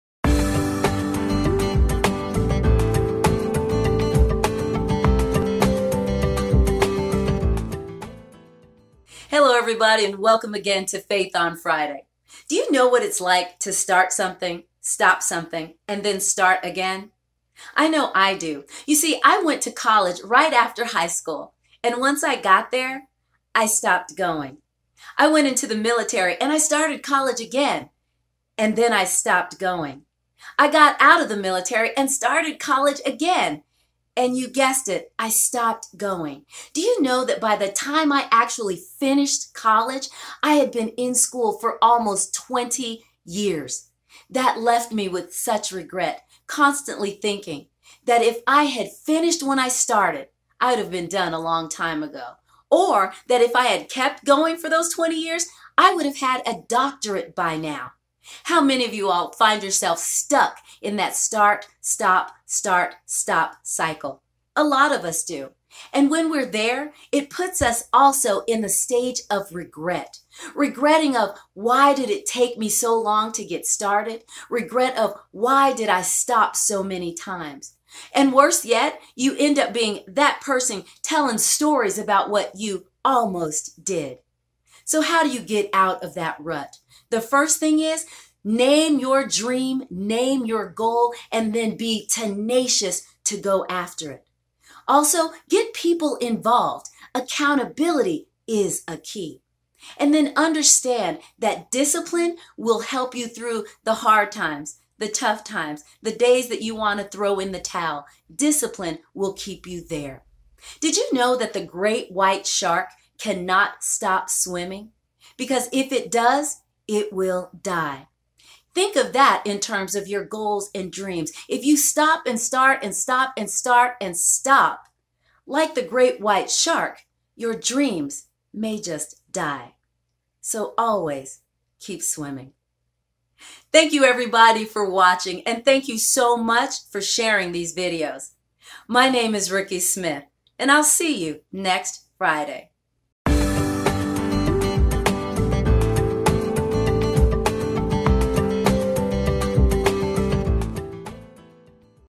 Faith on Friday - Start & Stop
The cycle of starting and stopping over and over can keep you from reaching your goals, and realizing your dreams. How do you get out of that rut? Like the great white shark; just keep swimming